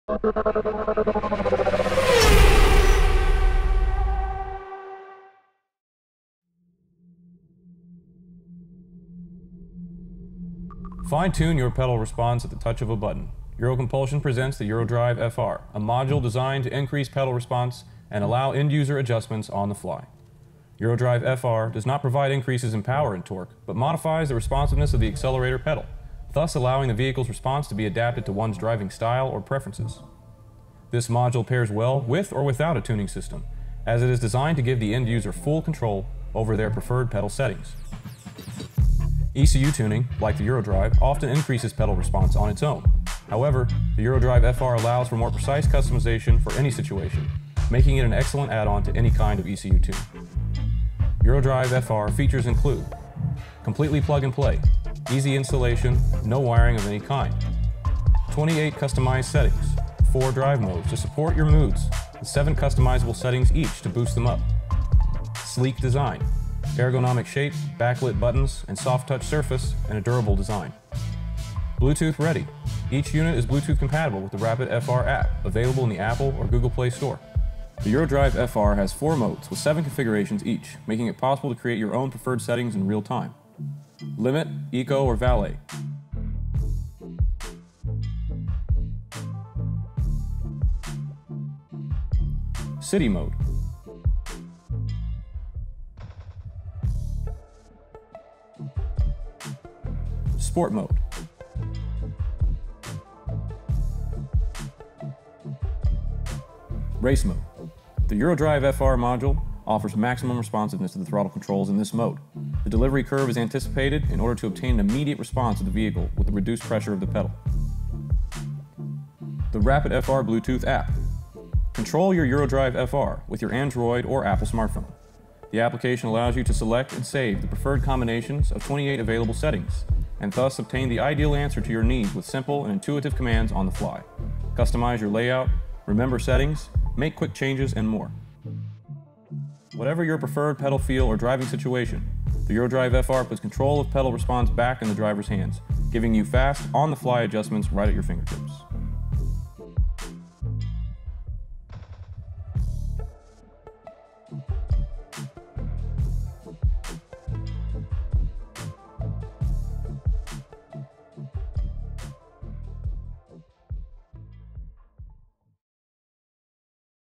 EURO+DRIVE (FR) Pedal Controller | EUROCOMPULSION
Fine tune your pedal response at the touch of a button! EUROCOMPULSION presents the EURO+DRIVE FR (Fast Response), a module designed to increase pedal response and allow end user adjustments on the fly.

 EURO+DRIVE FR does not provide increases in power and torque, but modifies the responsiveness of the accelerator pedal, thus allowing the vehicle's response to be adapted to one’s driving style or preferences.

This module pairs well with or without tuning systems, as it is designed to give the end user full control over their preferred pedal settings. ECU tuning (like the EURO+DRIVE) often increases pedal response, however the EURO+DRIVE FR allows for more precise customization for any situations, making it an excellent add on to any kind of ECU tune.

FEATURES:
ALL-IN-ONE MODULE: Single, double, triple analog, Pwm and Sent signals in a single unit.
PLUG & PLAY: Easy installation, no wiring.
28 CUSTOMIZED SETTINGS: 4 drive modes to support your moods + 7 customizable settings to boost them up
BLUETOOTH READY: Each unit is bluetooth compatible with the RAPID FR APP, available in the Apple or Google Play Store
SLEEK DESIGN: Ergonomic shape, back lit buttons, soft touch surface, durable design